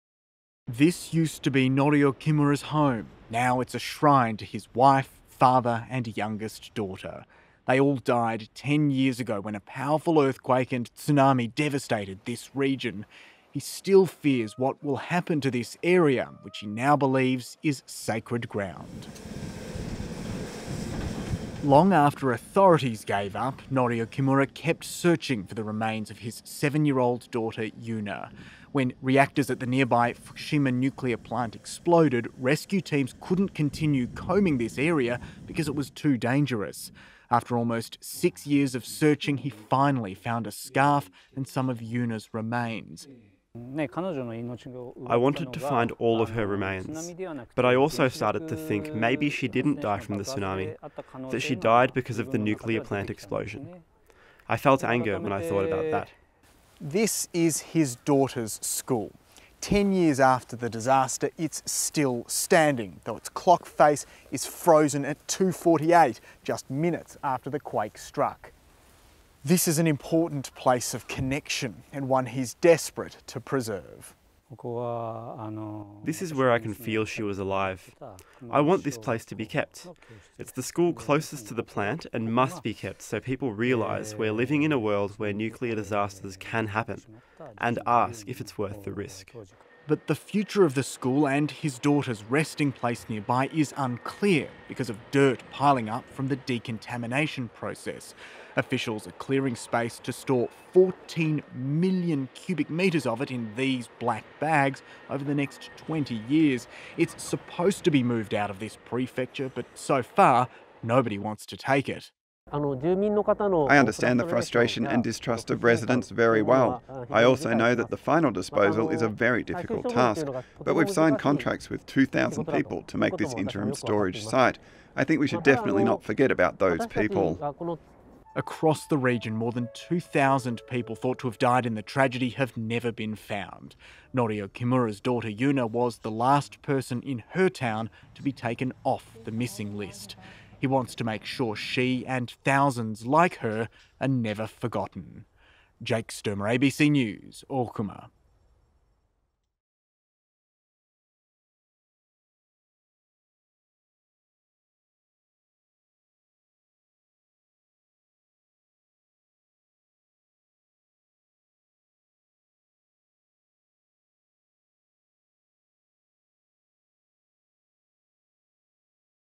A search for answers 10 years after the Fukushima earthquake | ABC News
Long after rescue teams gave up searching for survivors from the March 2011 tsunami, farmer Norio Kimura continued to painstakingly dig for his seven-year-old daughter, Yuna.